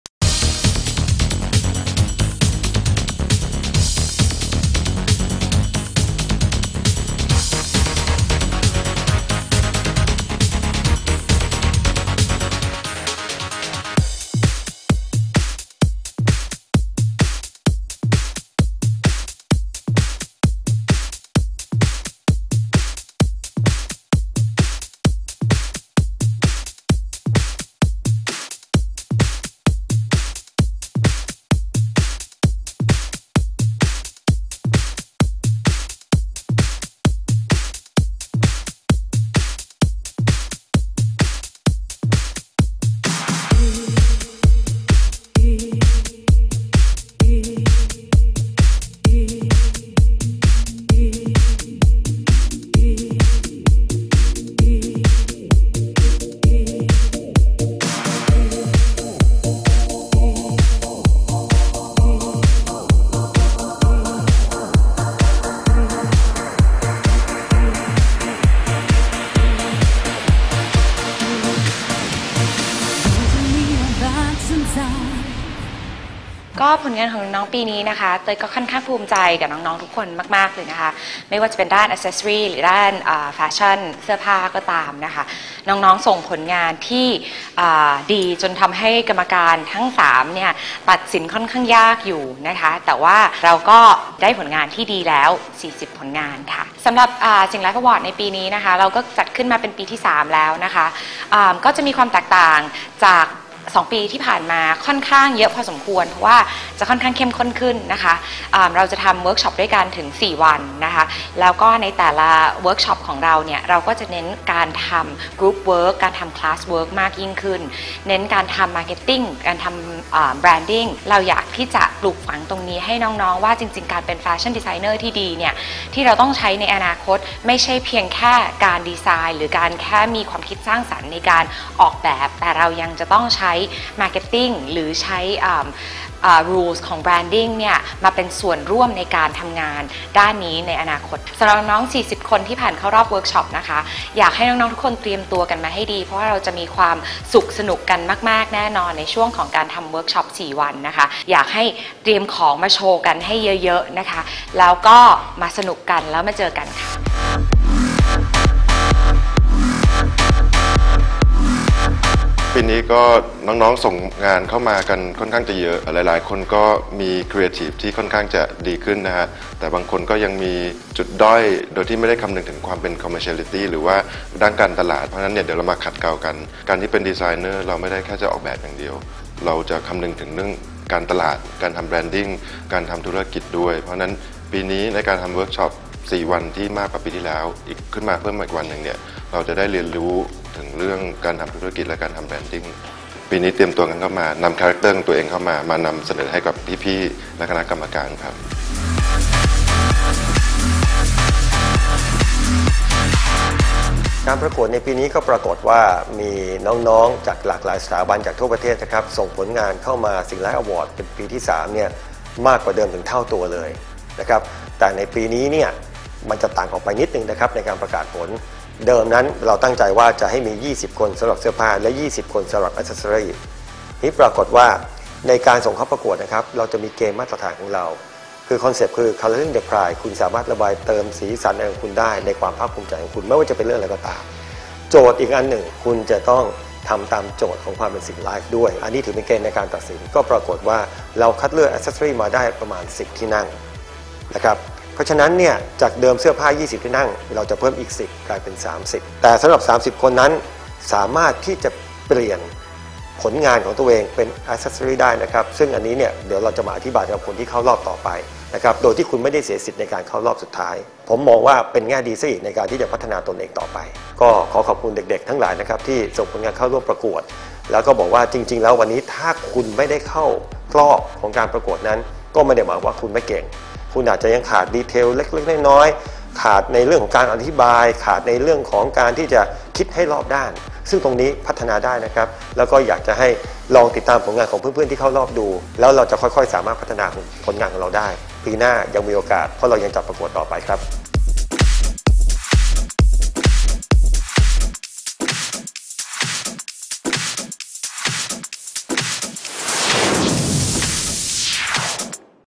[Singha Life Awards 2012] The Sketch Round
บรรยากาศการตัดสินของกรรมการในรอบ The Sketch (รอบแรก)